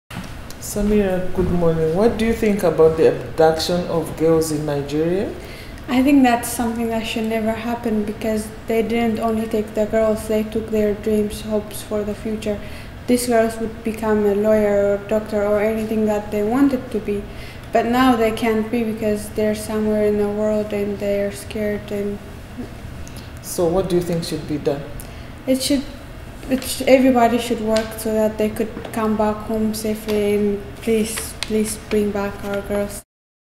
BringBackOurGirls
Emily Sikazwe speaks with Semira Abdu, a first-year student at St. Francis Xavier University in Antigonish, Nova Scotia Canada, about the recent kidnapping of more than 200 Nigerian schoolgirls. Sikazwe is a graduate of StFX and Coady International Institute, and was recently awarded an honourary doctorate degree.